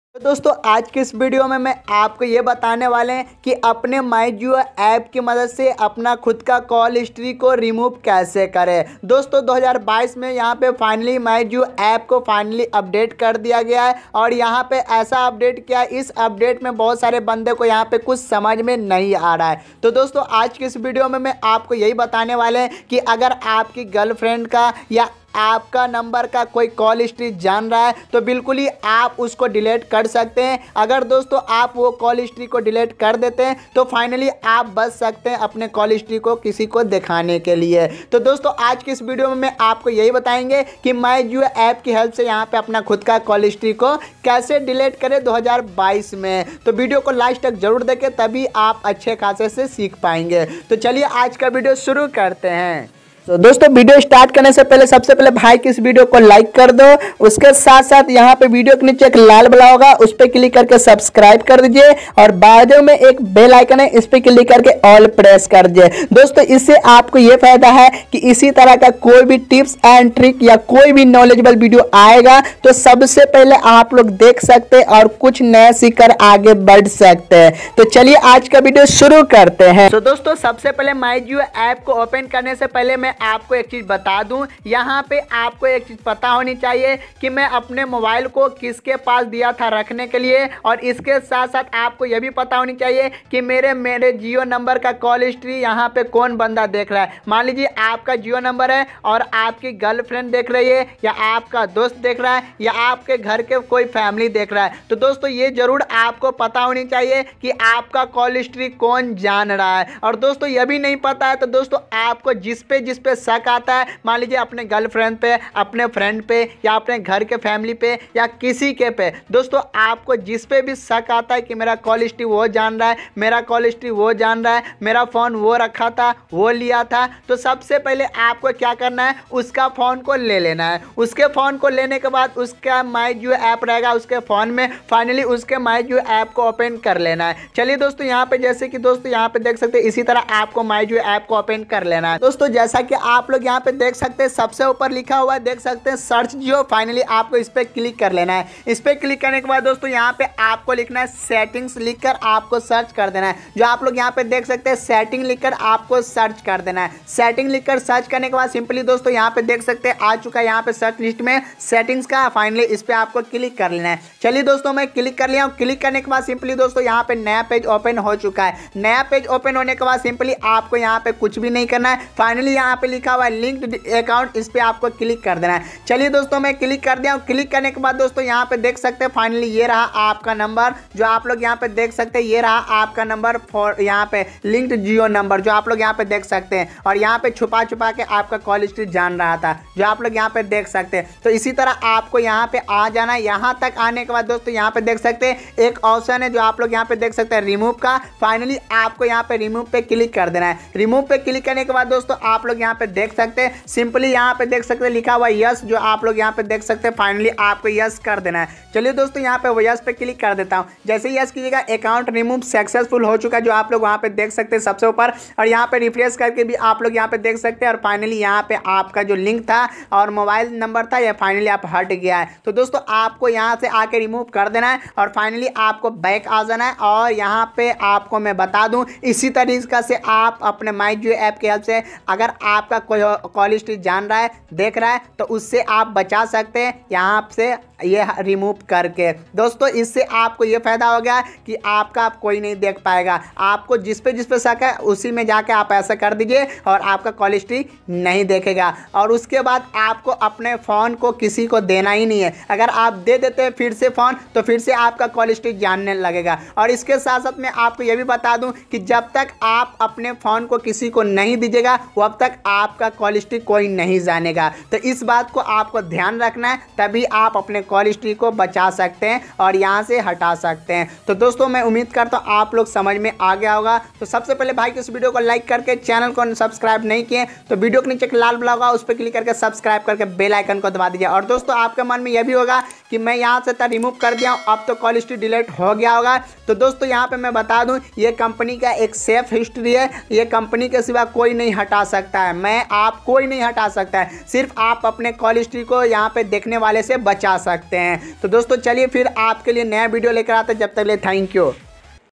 My Jio App Se Call History Remove Kaise Kare 2022 | jio call history kaise delete kare 2022
Swagat hai aap sabhi ka hamare YouTube channel me dosto Aaj ke Is Video me Bataya hu my Jio aap se call history delete kaise kare
Video ko last tak jarur dekhe Video Pasand Aaye To Like Kare aur Comment kare

WhatsApp ka Best Trick Aur Jadu

My Jio App se call details kaise Nikale